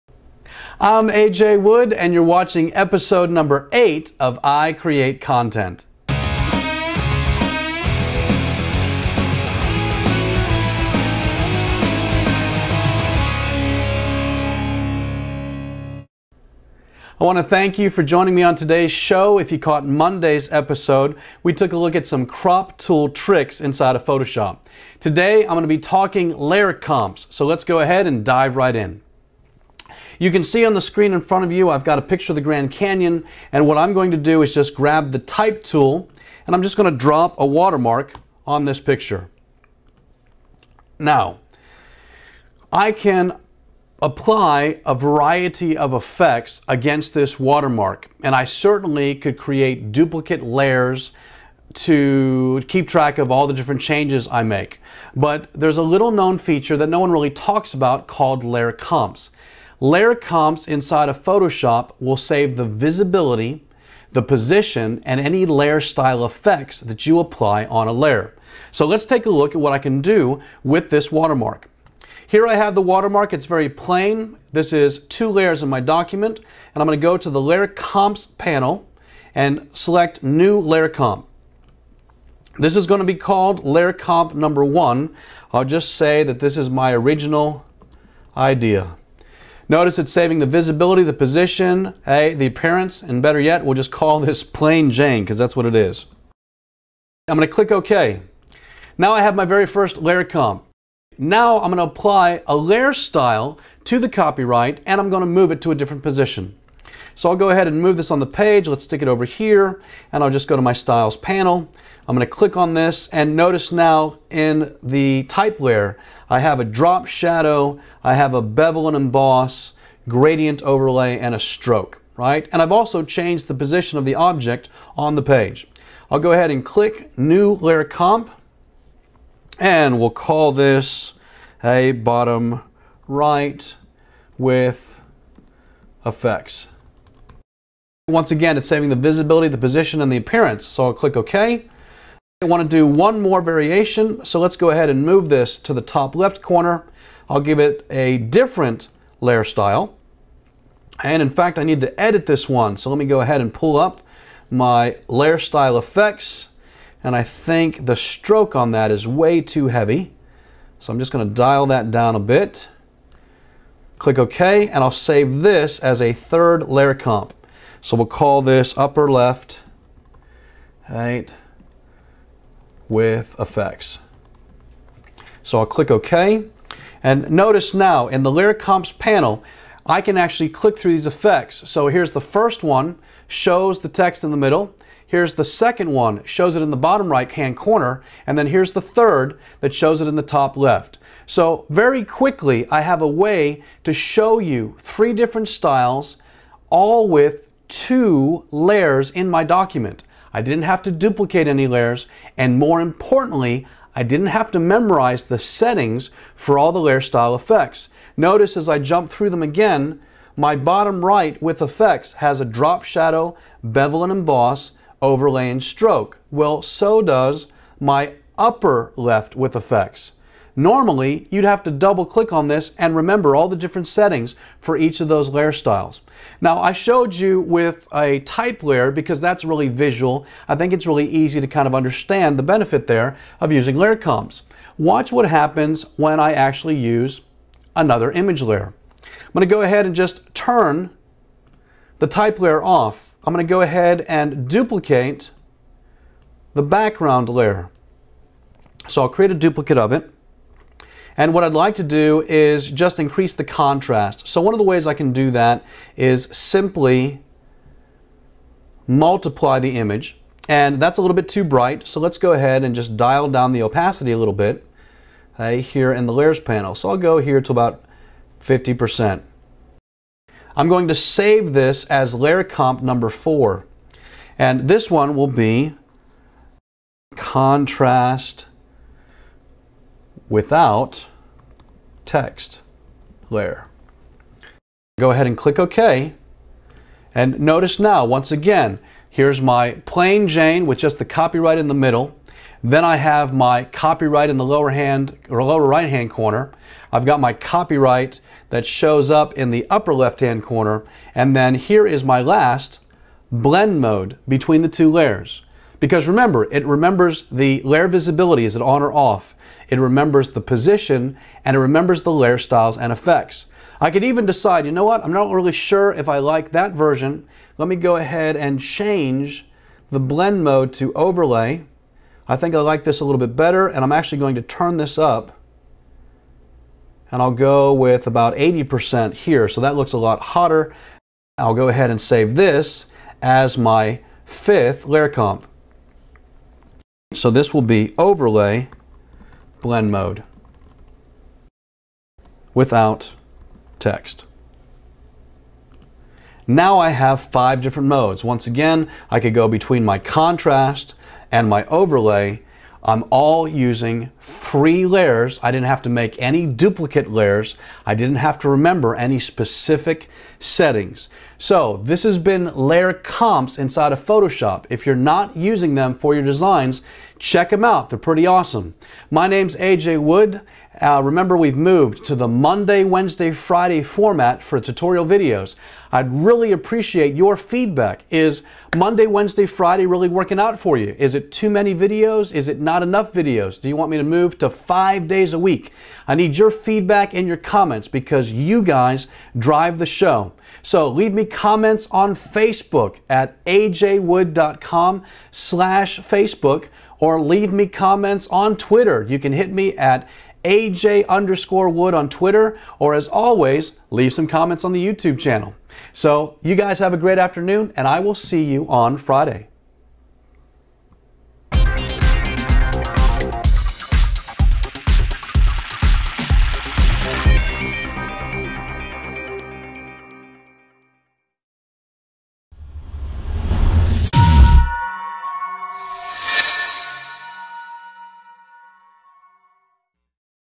How To Use Layer Comps to Organize Designs in Photoshop
- I Create Content #8. Learn how to organize your designs using Layer Comps in Photoshop.

Nation Association of Photoshop Professionals
NAPP Wants YOU!